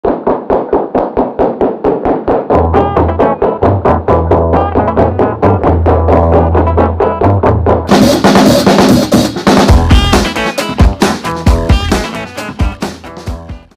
Crooks 1926 Trailer
Plunge into the cockney criminal underworld of Elephant and Castle as you witness one family’s brutal battle for total supremacy. Nothing is certain and bargains are made to be broken. In COLAB’s new, dedicated immersive space with its most ambitious production yet, you decide the outcome of all...